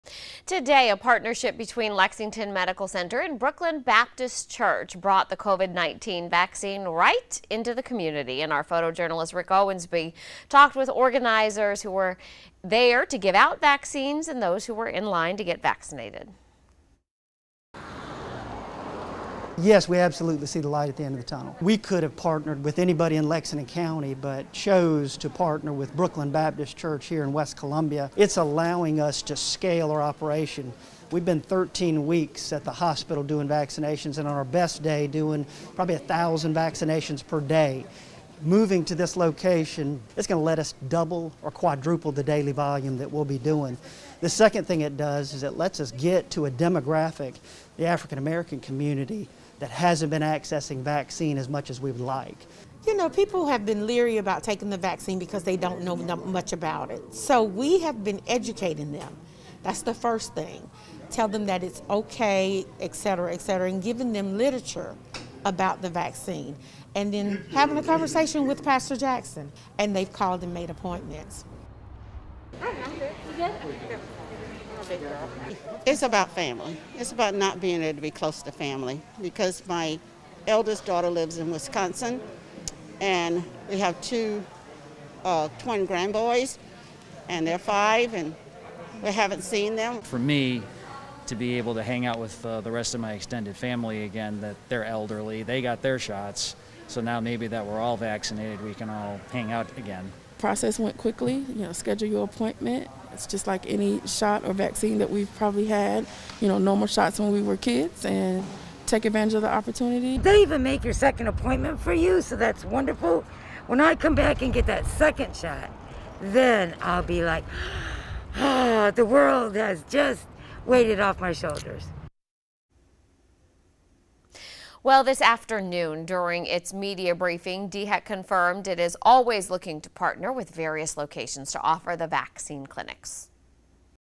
Lexington Med and Bookland Baptist hold vaccine clinic
Lexington Medical says this site will allow them to be able to do three times as many vaccines.